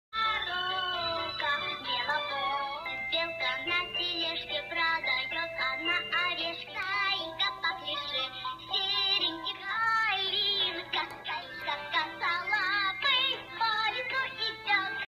Плеер Потешка код 000-0098947
Плеер поет веселые песенки и рассказывает о лесных животных.